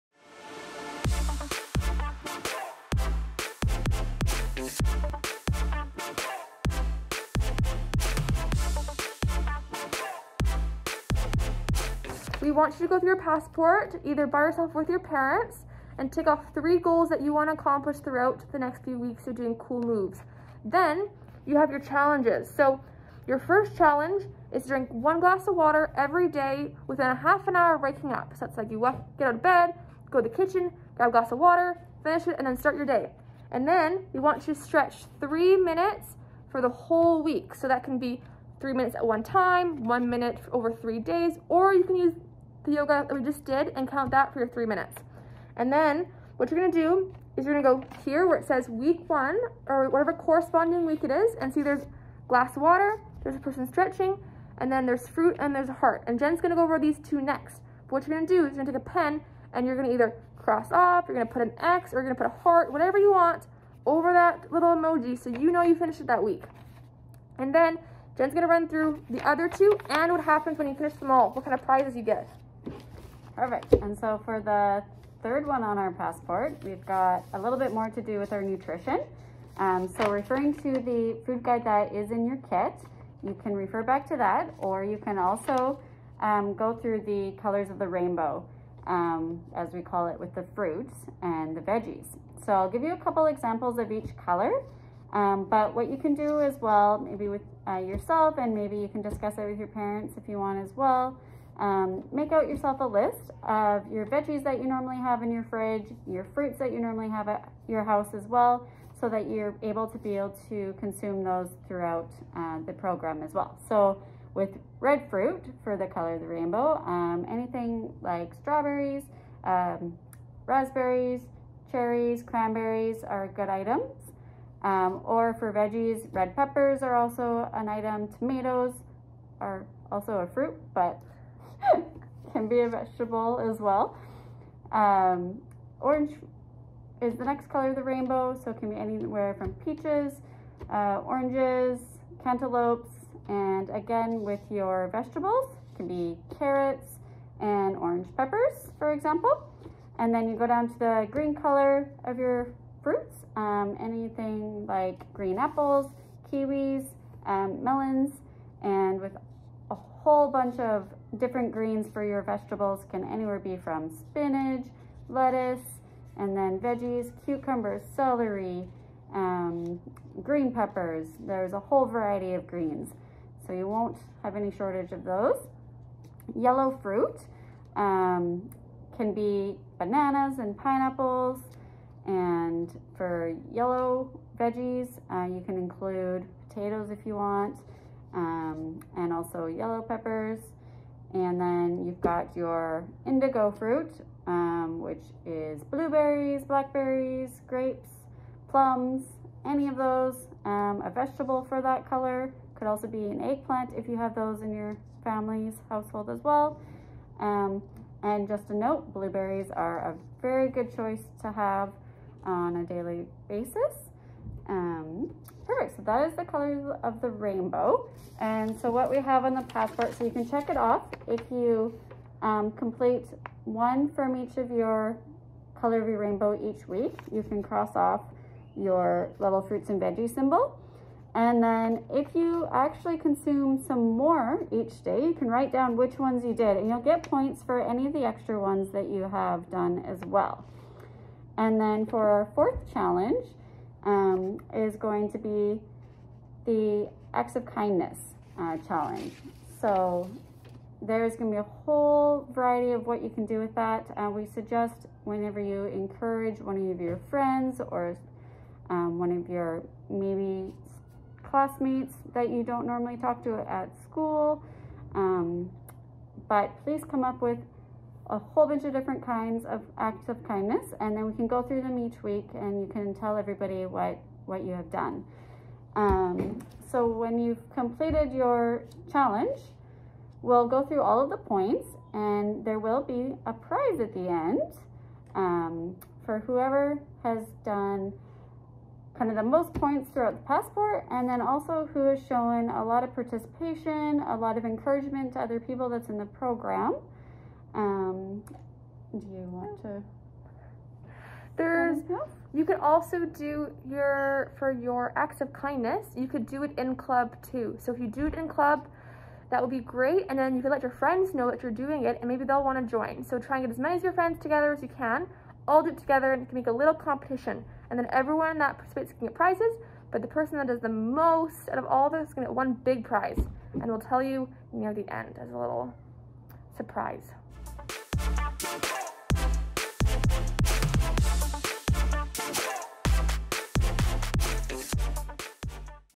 BGCO Cool Moves - Week 1 Challenge
Cool Moves is a virtual program run by the Boys and Girls Clubs of the Okanagan. Follow the Week 1 video and then try your best to complete the Week 1 challenge.